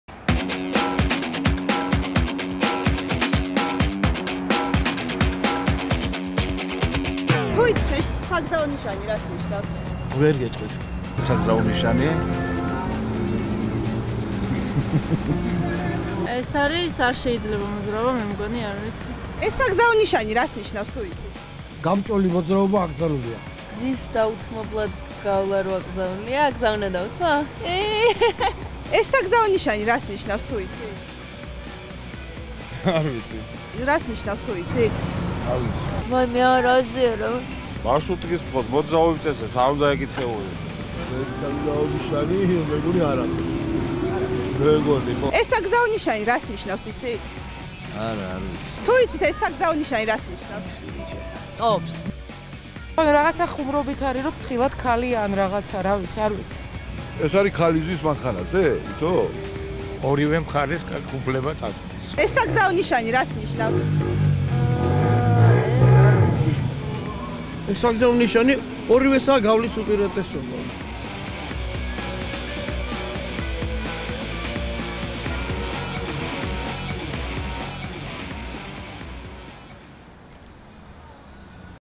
მძღოლები და საგზაო ნიშნები
2014 წლის 15 დეკემბრიდან მართვის მოწმობის ასაღებად საჭირო თეორიული ტესტი იცვლება